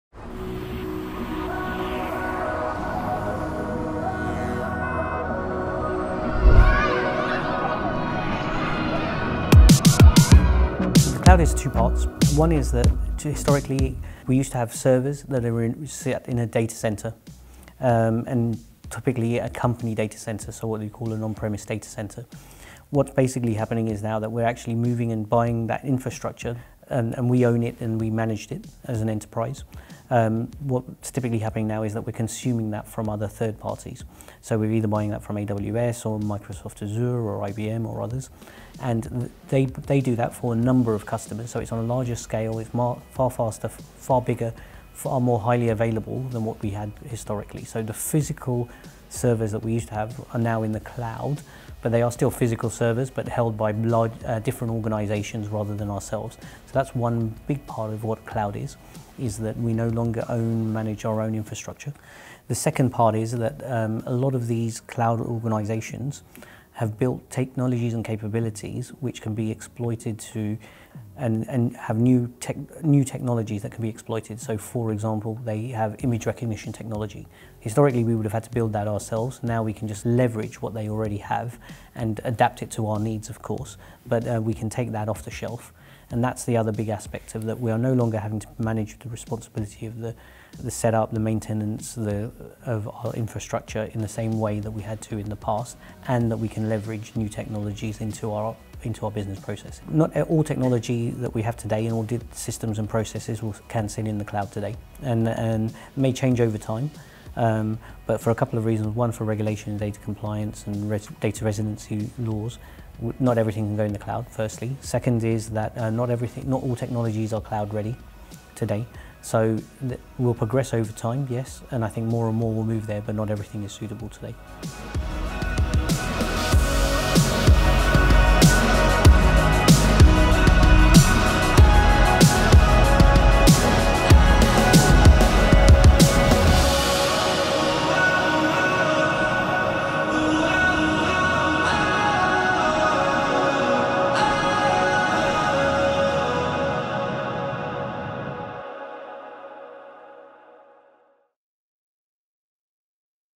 What is the Cloud? | AXA Global Program Director, Ash Shah | FinTech
In January, the team at FinTech Magazine travelled to Paris to speak to Ash Shah, the Global Program Director for AXA's Move to the Cloud! In this video Ash gives a detailed explanation of what the cloud is and why it's important.